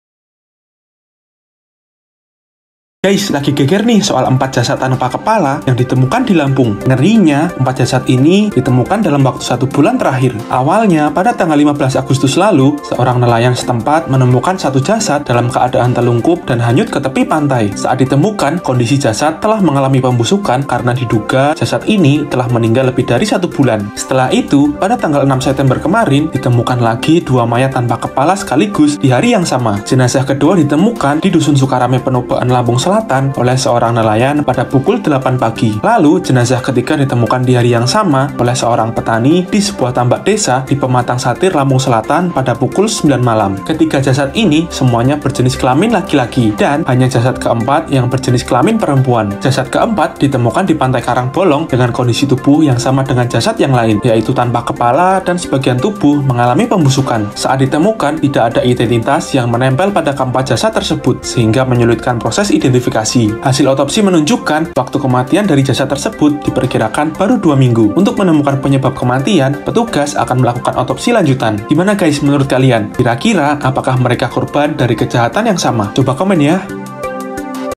Misteri Temuan Beruntun 4 Jasad Tanpa Kepala di Lampung
Dalam kurun waktu hampir satu bulan, sebanyak empat jasad anonim ditemukan di perairan Provinsi Lampung.
Empat jasad ini memiliki kondisi yang sama yakni bagian kepala hilang. Jasad pertama ditemukan di Pantai Cukuh Gayau, Kabupaten Tanggamus pada 15 Agustus 2023 kemarin.
Kapolsek Pematang Sawa Inspektur Dua (Ipda) Arif Janto mengatakan saat ditemukan jasad itu dalam keadaan menelungkup dan terhanyut ke tepi pantai.
Kemudian, penemuan jasad kedua dan ketiga terjadi dalam satu hari yang sama yaitu pada Rabu (6/9/2023).
Sedangkan penemuan terkini terjadi di Pantai Karang Bolong, Pekon (desa) Tegineneng, Kecamatan Limau, Kabupaten Tanggamus pada Kamis (7/9/2023) kemarin.
Kapolsek Limau Iptu Dediyanto mengatakan kondisinya mengambang tanpa kepala, telapak tangan dan kaki.
Video Editor: Ricky Arista
Produser: Nibras Nada Nailufar